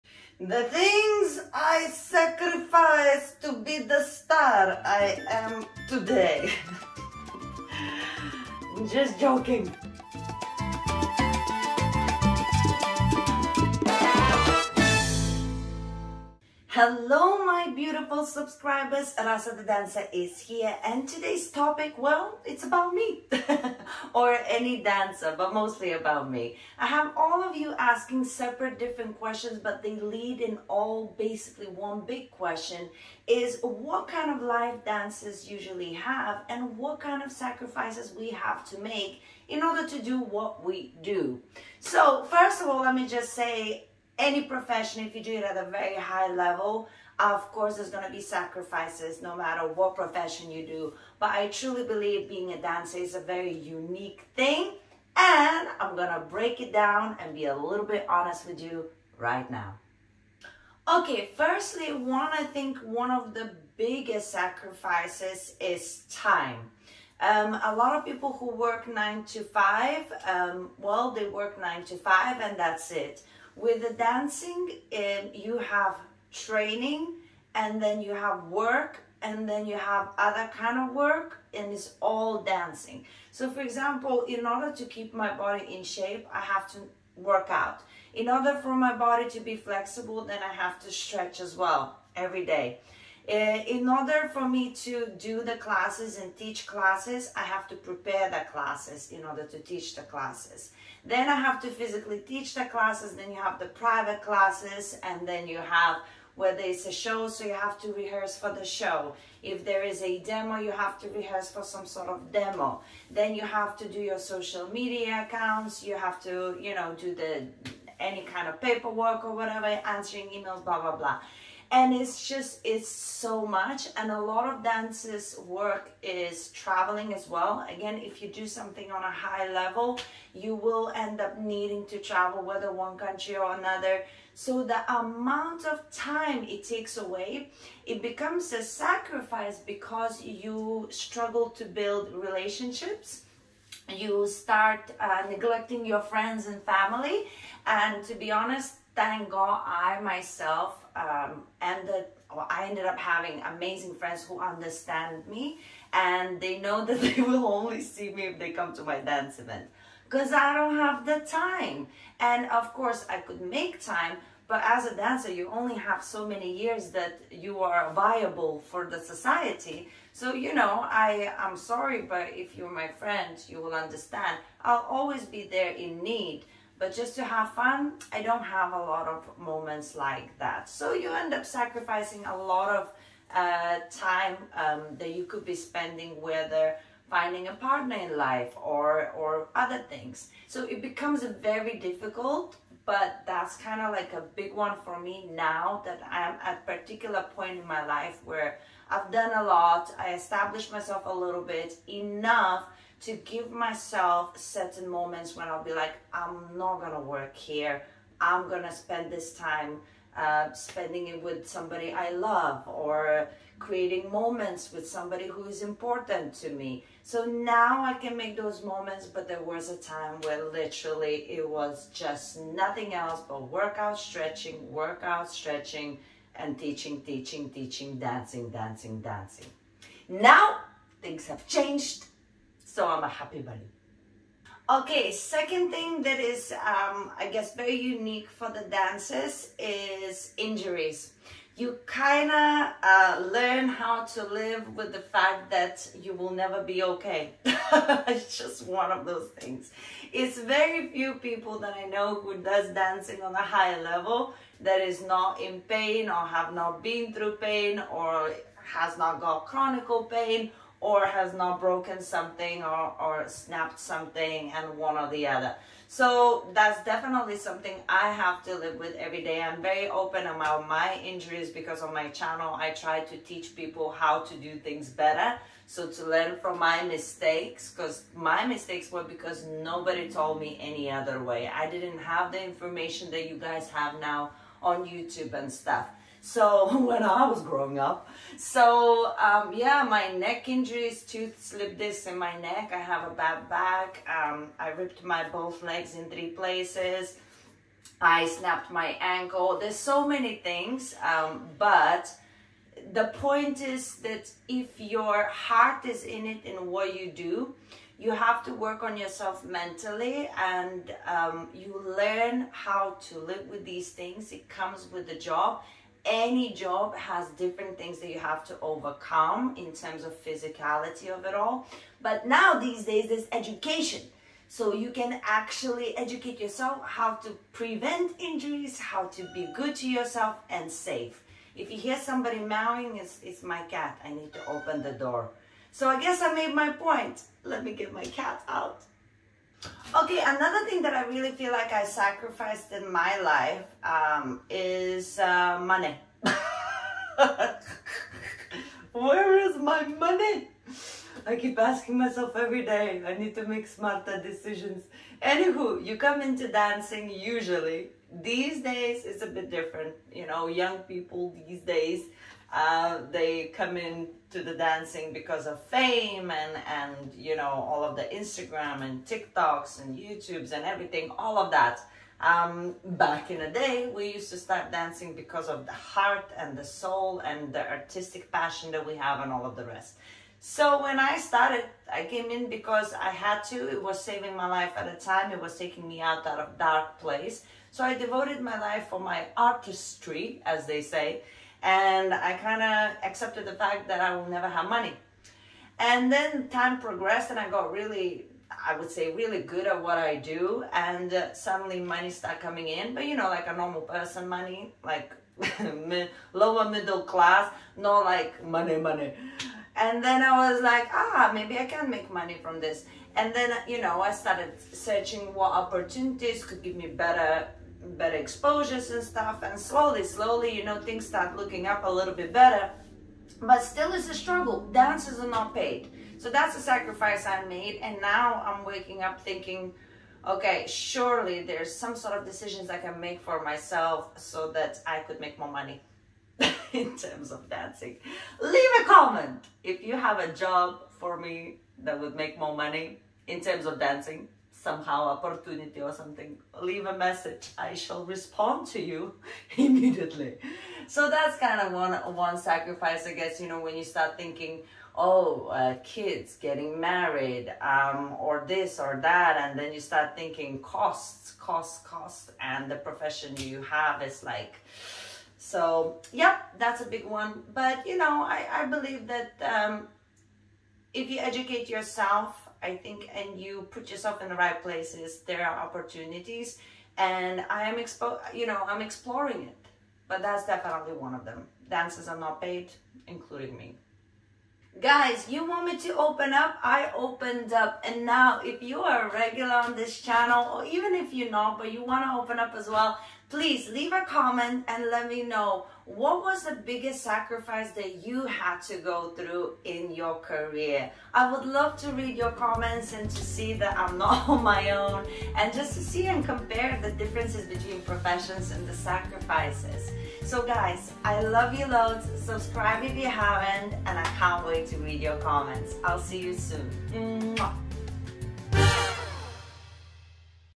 3 Things I Sacrificed To Become A Dancer - Dance With Rasa
Dancing is my life, but that doesn't mean that it came without sacrifices! Today I am giving you a little bit of an insight into my dance journey, telling you about some of the things I have had to sacrifice to make a success of my career as a dancer. Can you relate?

It makes me feel special and encourages me to share more of my life with you!